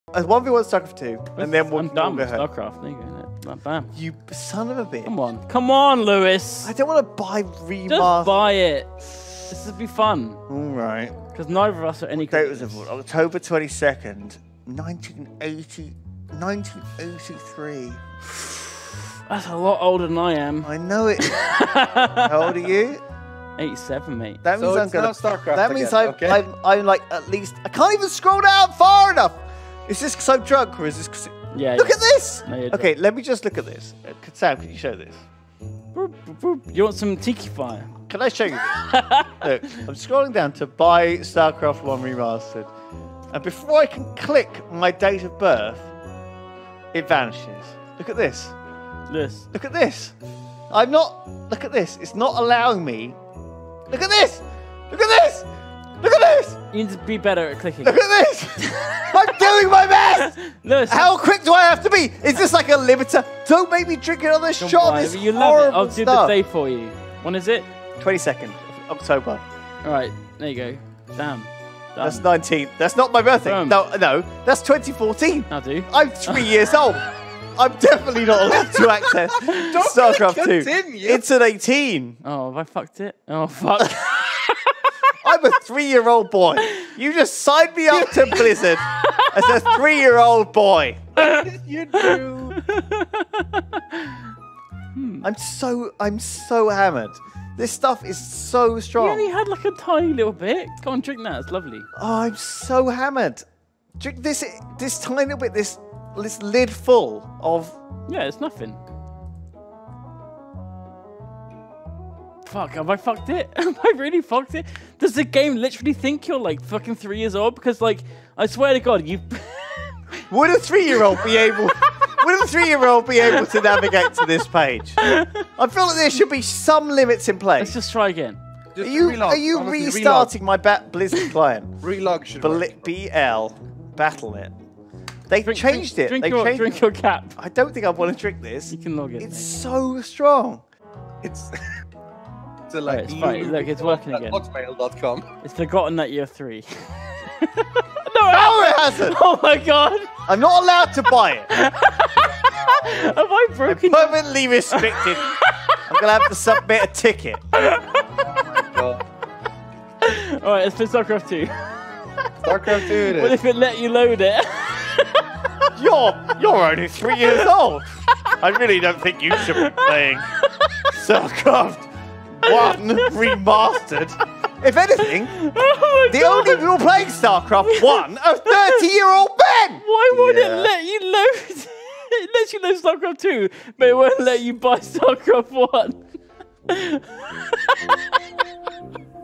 Duncan accidently trolls Lewis in the most hilarious way
One of my all time favourite clips that I found on my pc. I laugh every time I watch it.